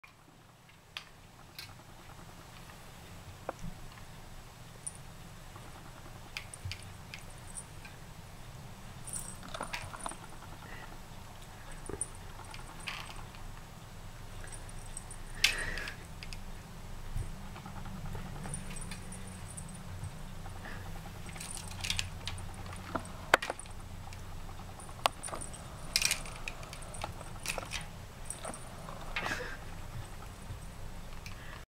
cat playing with remote control arial
Ava grew bored of the tiny remote control car quickly but realised that the arial on the remote control was far more entertaining :D